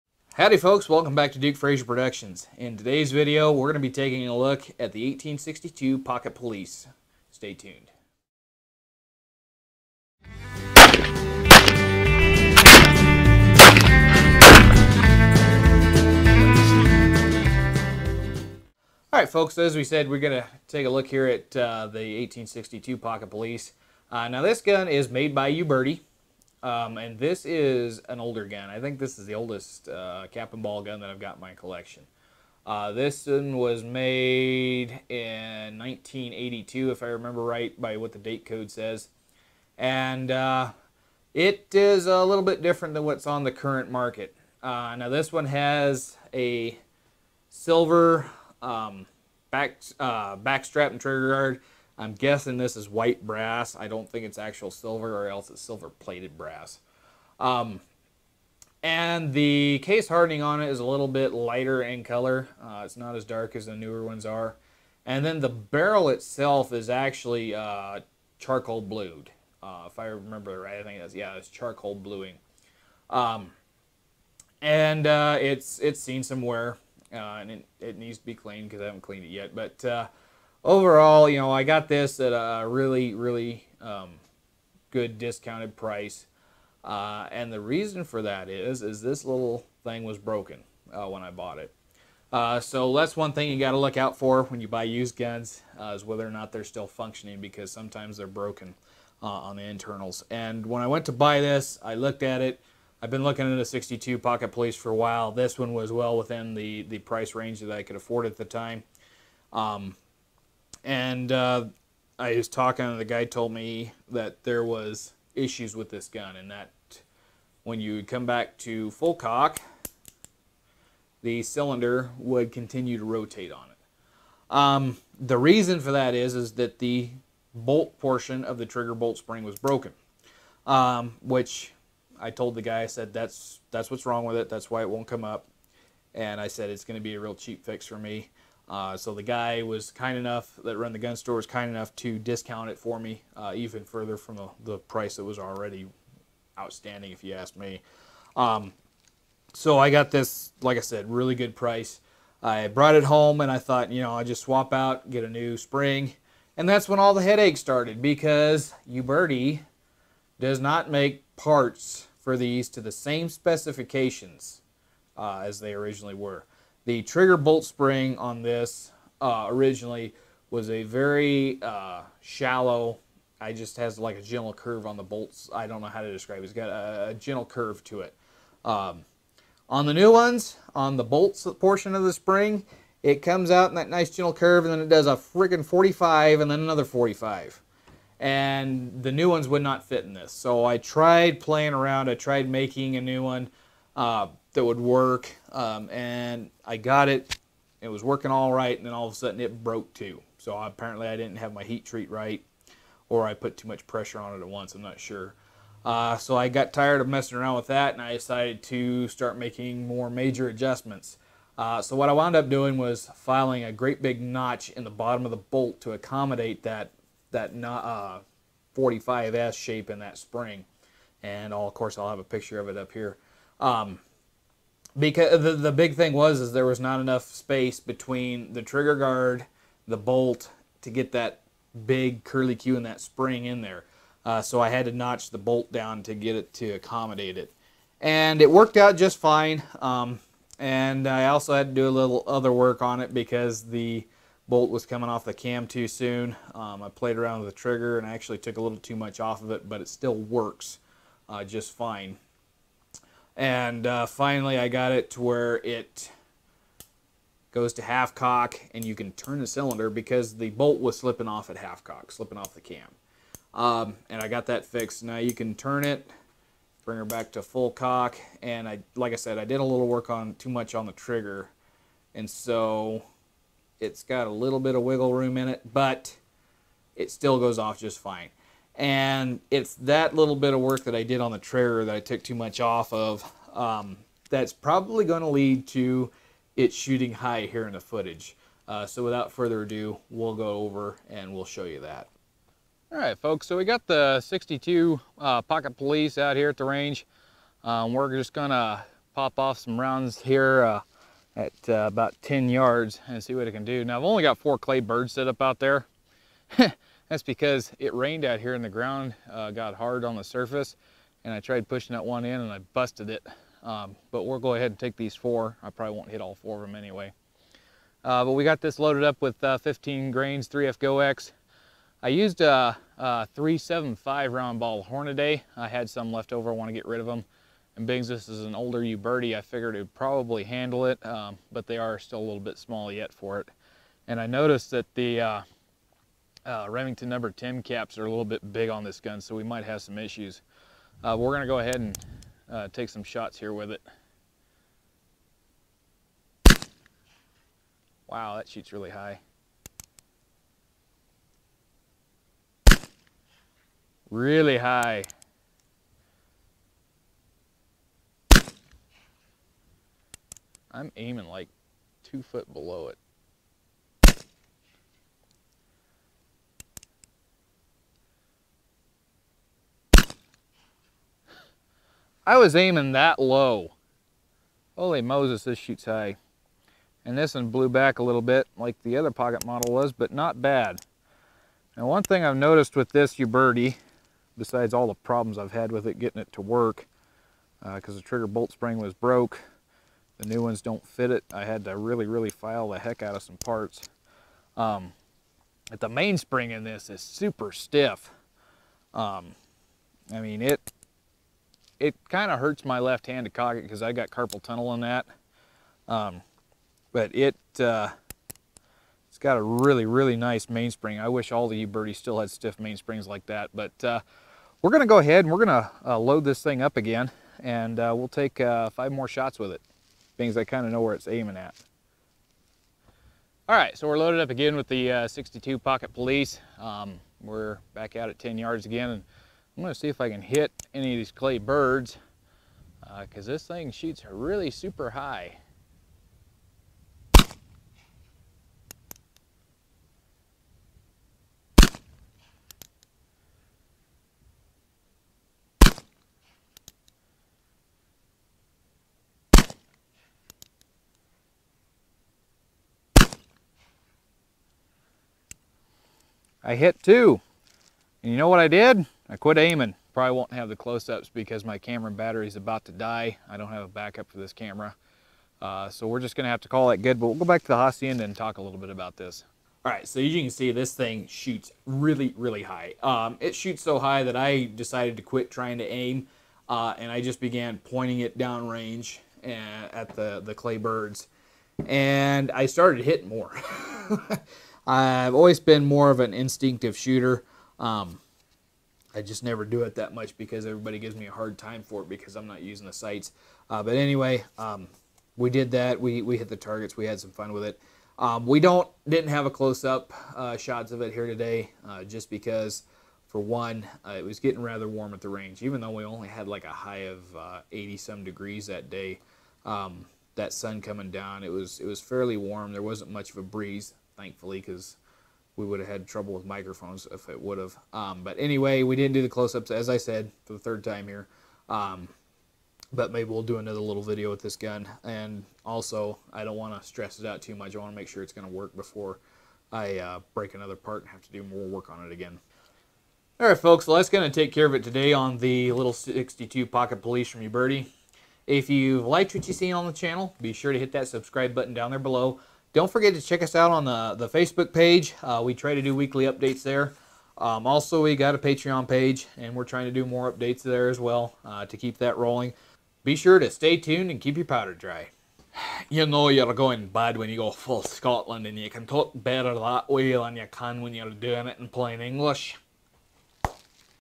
Uberti 1862 Pocket Police
In this video we will take a look at an older Uberti 1862 Pocket Police manufactured in 1982.